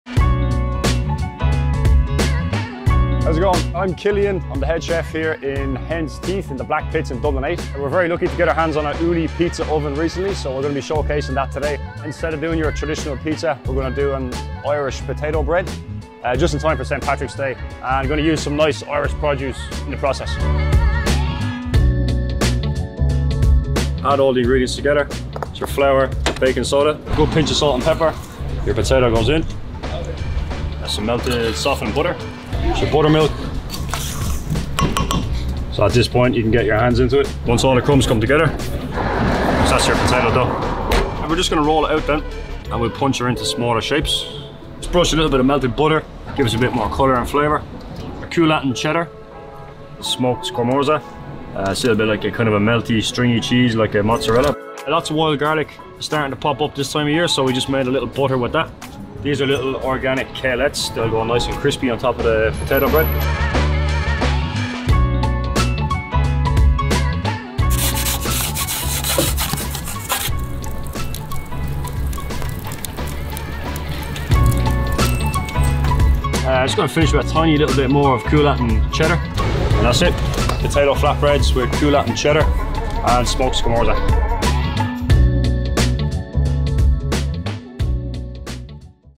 Potato Flatbreads with Garlic Butter, Cheese and Baby Kale | Ooni Pizza Ovens
We asked Greg Spring, founder and creative director of Hen’s Teeth Dublin and Head Chef Killian Walsh to create a unique recipe fit for St. Patrick’s Day celebrations. They landed on this cheesy potato farl (an Irish savoury pancake of sorts made with flour and potato).

Like the classic potato farl, Killian’s recipe uses grated potato and flour. It includes buttermilk for an extra-creamy texture and is topped with wild garlic butter, baby kale, smoked scamorza, and local Irish cheddar before getting fired in the oven. Killian recommends Coolattin cheddar, a local, clover-fed cow’s cheese from West Wicklow, but you can use your favourite aged cheddar if none is available near you. The garlicky butter, crisp kale, and melty cheese work together to create layers of irresistible flavour and texture. The resulting flatbreads are larger than a dough ball but smaller than a pizza, making them an ideal party food. These require no resting or fermentation, so they can be whipped up day-of and served to hungry guests within hours.

Hen’s Teeth is a café, store, gallery and creative studio in Dublin where love for great food, art, music and culture all come together. There, head chef Killian “Killer” Walsh (formerly of the Michelin-starred L’Enclume and Pied à Terre) creates contemporary Irish food with influences from his time spent in kitchens around the globe.

Ciaran Harte - DOP
Charlie Doran - Edit

Video content
​​00:00​​ - Introduction
00:24​​ - Make the dough
​00:36 - Knead the dough
00:42 ​- Roll out and shape the dough
00:45 - Add toppings
01:05 - Cook in Ooni pizza oven
01:14 - Final touches and serve!